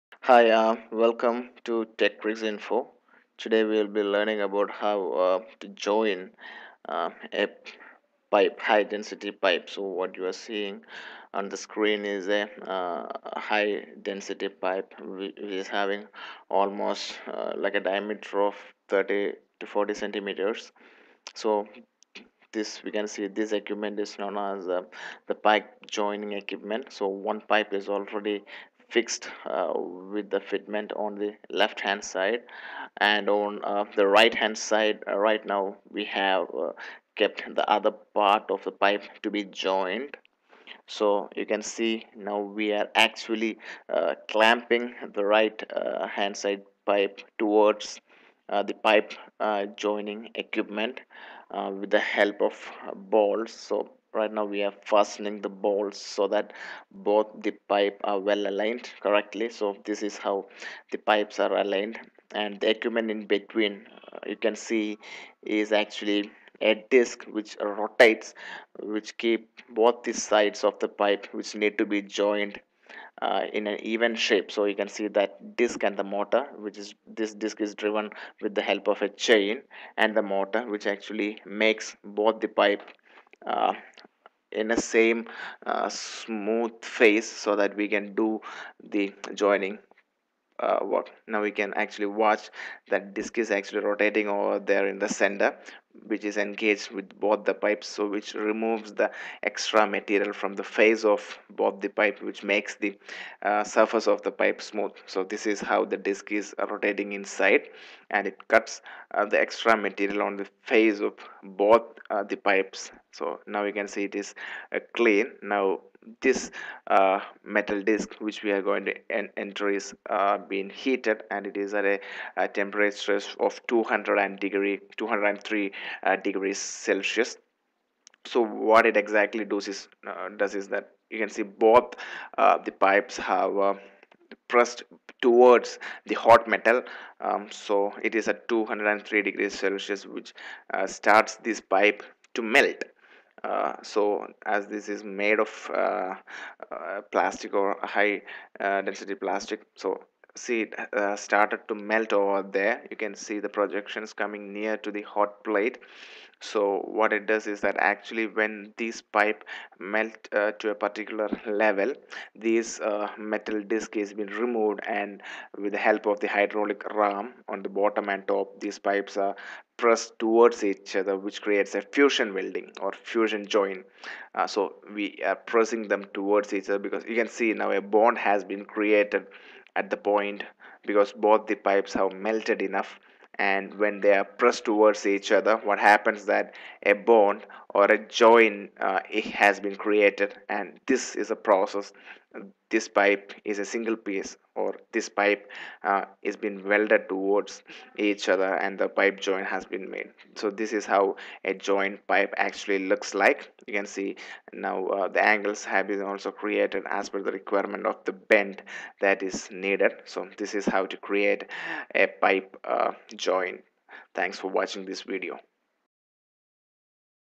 How fusion welding on high density pipe works. ✔
How fusion welding joining a high density pipe.
This video explains the process of fusion welding done on high pressure pipes. At first the pipes which need to be joined are brought together and the faces are made clear and parallel to each other as shown in the video (the cutting equipment which rotates does this job). Now both the faces are been heated to about 203 degree Celsius. After the recommended time period of 5 minutes, the heated bar is removed. At this stage the pipes will be in melted state. Now they are been joined with pressure using the hydraulic ram of the equipment.
With in one minute time the pipes get joined, and the process is finished.